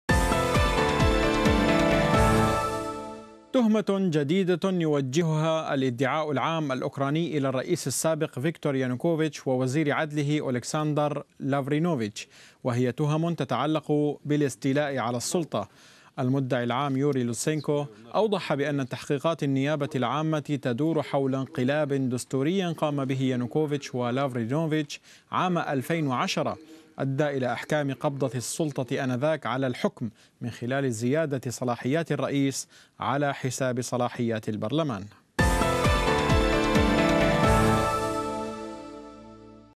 يانوكوفيتش متهم بانقلاب دستوري في 2010
تهمة جديدة يوجهها الادعاء العام الأوكراني إلى الرئيس السابق فيكتور يانوكوفيتش ووزير عدله أوليكساندر لافرينوفيتش، وهي تهم تتعلق بالاستيلاء على السلطة.

المدعي العام يوري لوتسينكو أوضح بأن تحقيقات النيابة العامة تدور حول انقلاب دستوري قام به يانوكوفيتش ولافرينوفيتش عام 2010، أدى إلى إحكام قبضة السلطة آنذاك على الحكم، من خلال زيادة صلاحيات الرئيس على حساب صلاحيات البرلمان.